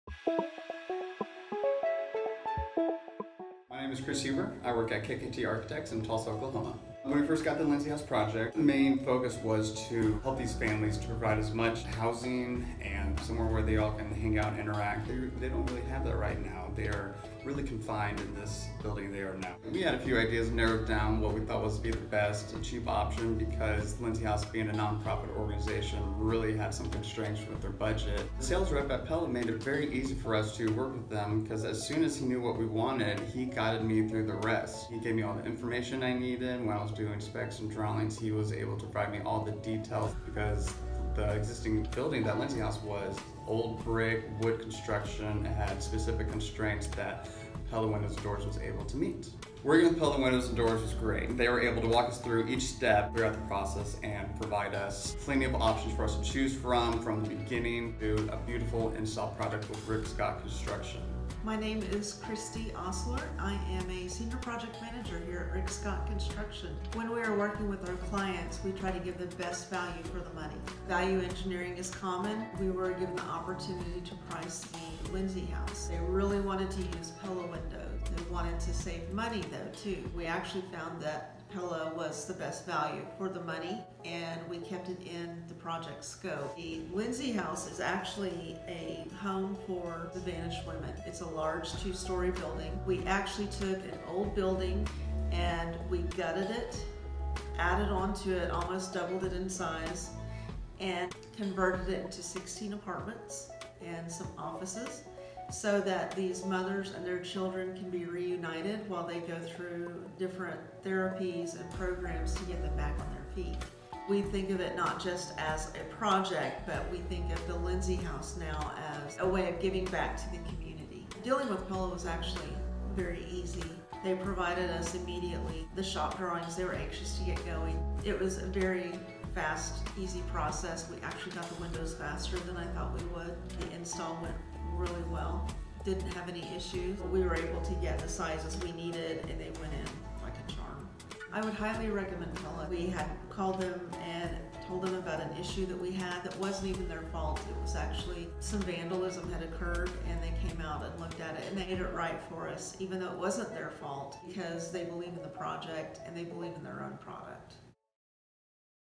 Commercial Project - Lindsey House
Lindsey House is in Tulsa, OK and helps women and children transition out of homelessness.